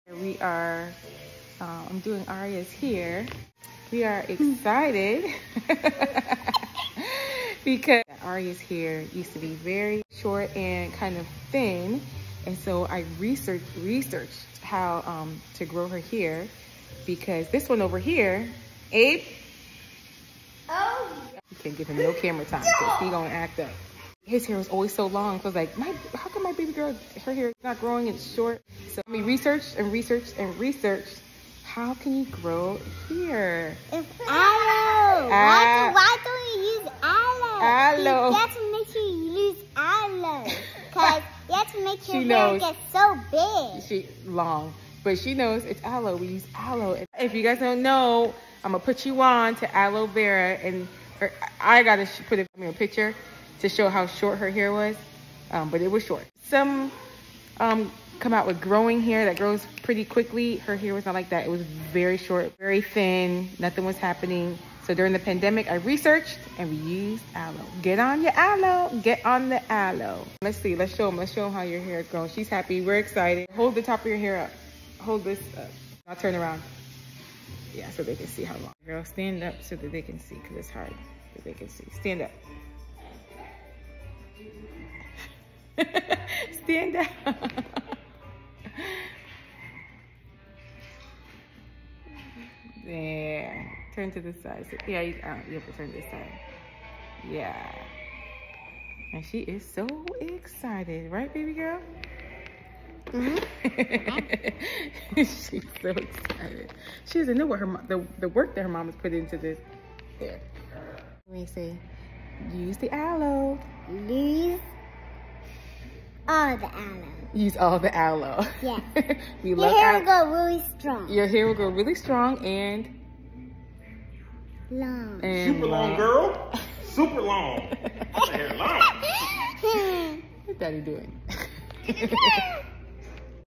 Aloe Vera = Hair Growth WIN!
For the last 3 years we’ve been using Aloe Vera to grow Aria’s hair. And it has grooooown!